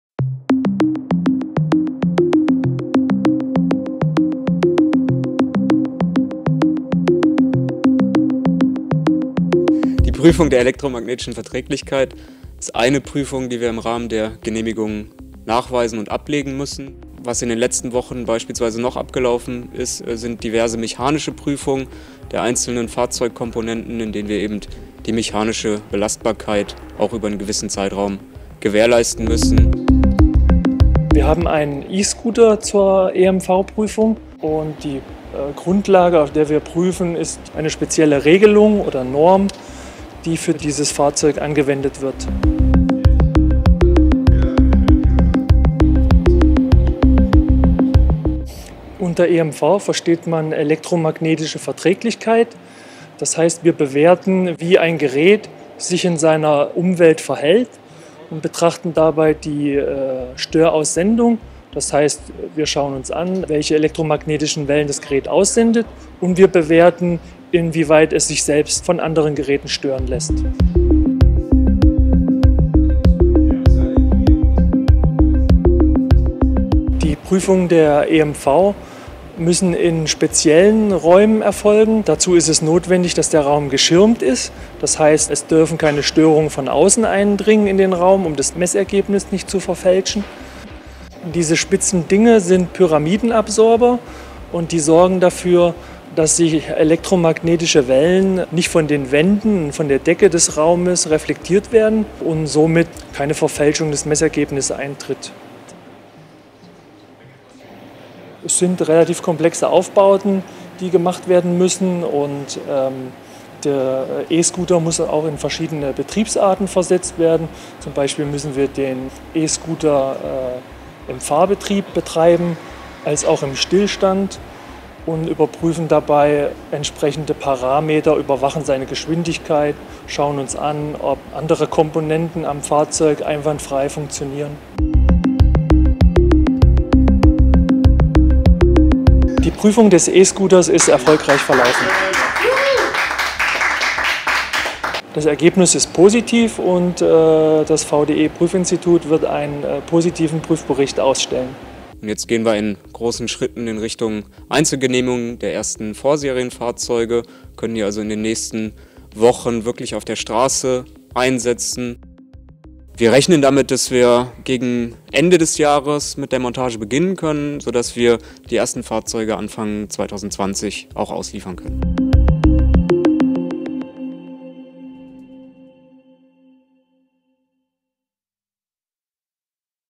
EMV-Prüfung des E-Scooters STEEREON im VDE-Institut
Ein Blick in die Absorberhalle und das Prüfverfahren: Der E-Scooter "STEEREON" des VDE e-diale Zukunft Challenge Siegerteams von PLEV Technologies aus Köln stand auf dem EMV-Prüfstand des VDE Prüf- und Zertifizierungsinstituts. Untersucht wurde die elektromagnetische Verträglichkeit (EMV) des E-Scooters. Ein weiterer erfolgreicher Schritt bis zur endgültigen Straßenzulassung und Serienfertigung.